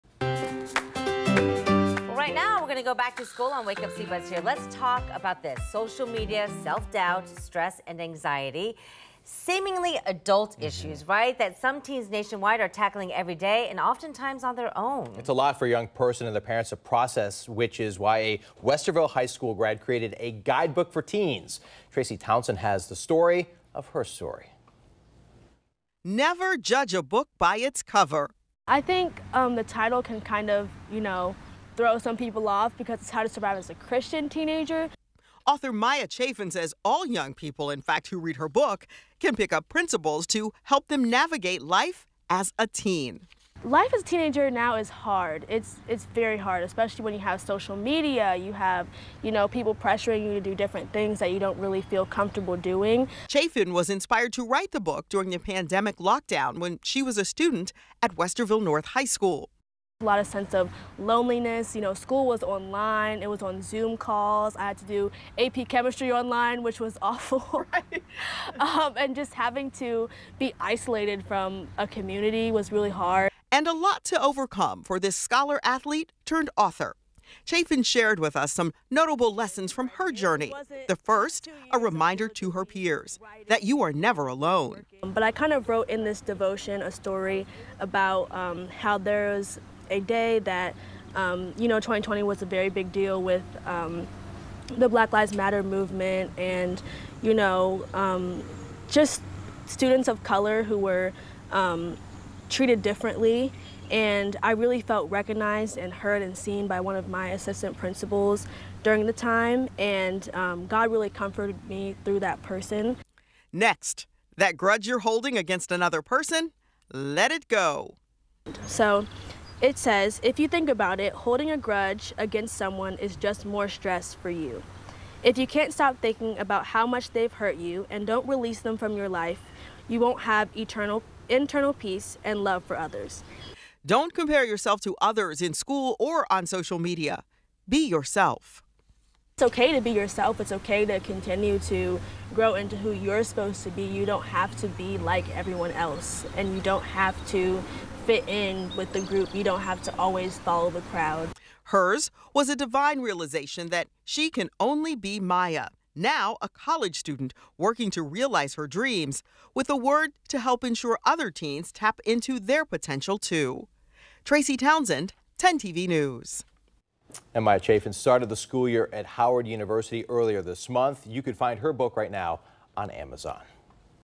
Westerville North graduate writes book to help young adult navigate life as a teenager
Author Maya Chaffin said all young people who read her book can pick up principles to help them navigate life as a teenager.